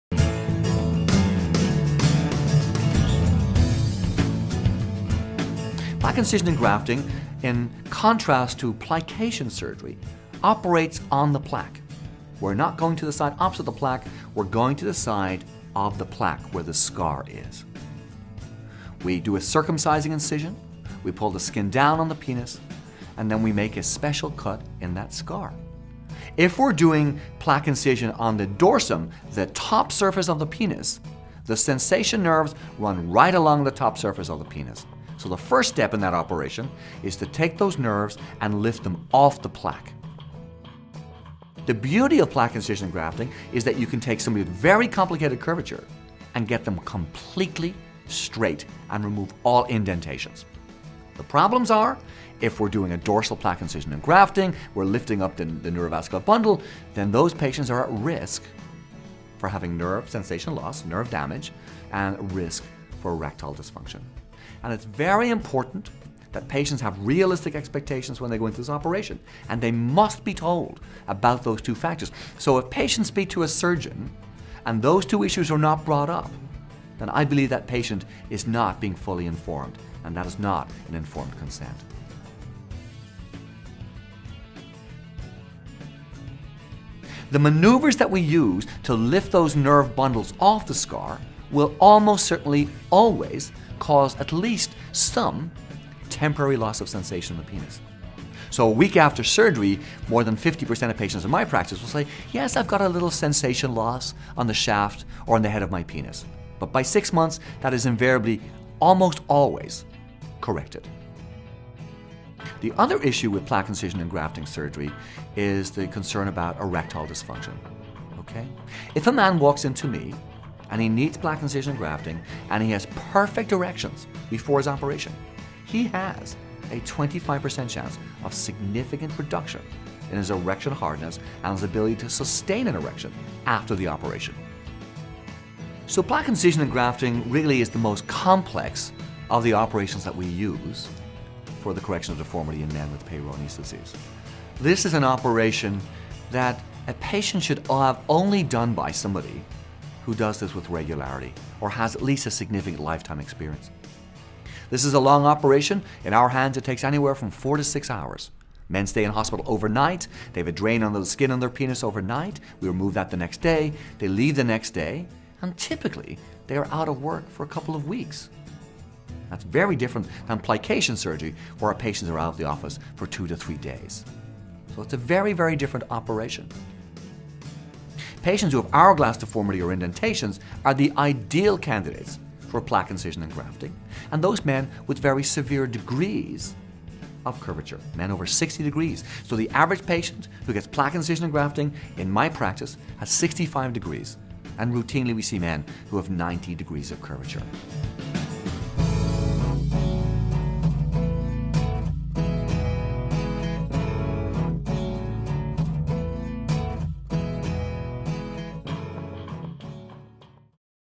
Plaque Incision & Grafting for treatment of Peyronie's Disease
Dr Mulhall of Memorial Sloan Kettering Cancer Center discusses the Benefits and Risks of Plaque incision and Grafting surgery for the Peyronie's patient.